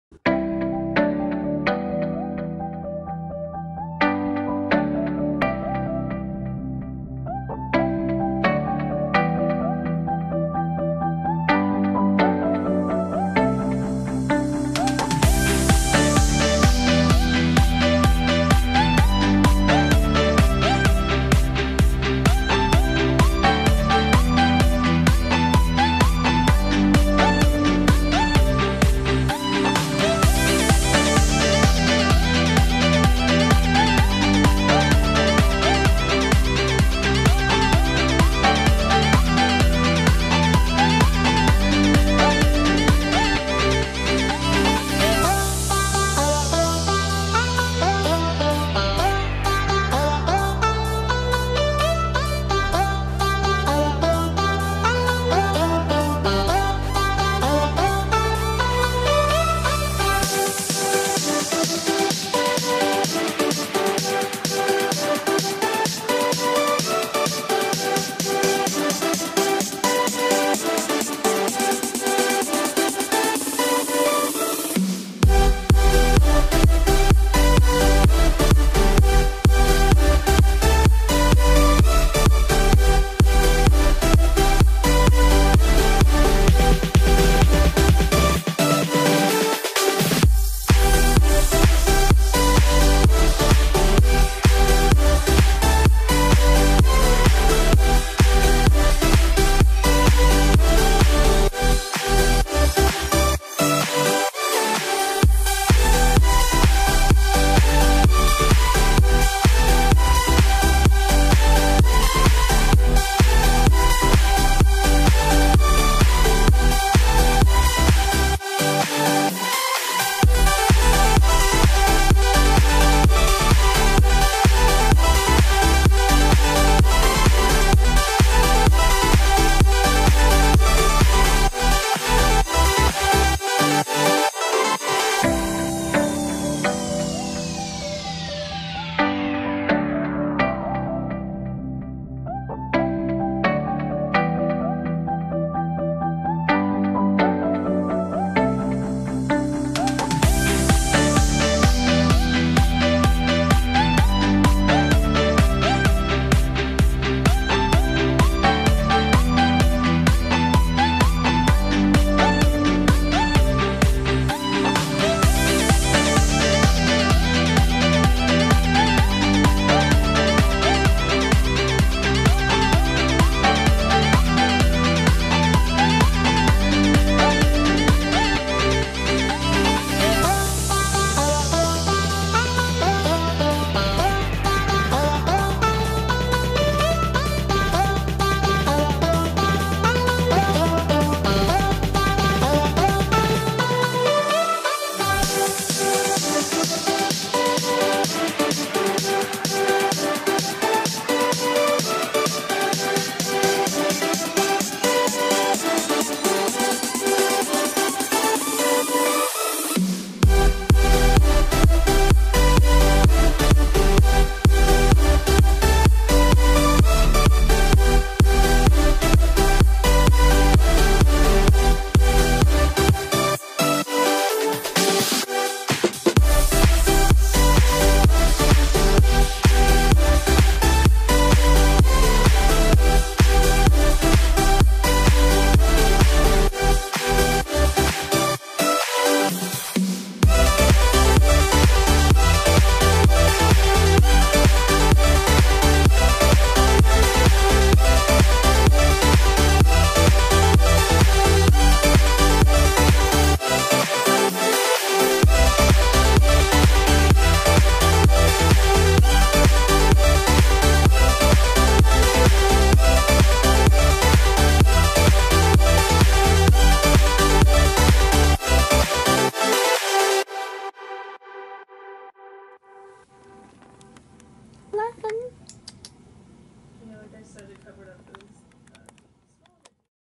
Freaking art challenge that I thought I was able to take on.
I usually draw really quickly, but for some reason I was slower then usual.
Besides that, it was an interesting experiment.
Character's name is Bon, she is my angelic Kitsune-Bunny mix, and shes like the easiest character I have besides my Polar bear.

Also, at the end, there is my sweet furbaby Len. I love him so much ^O^ Maybe I'll draw him next?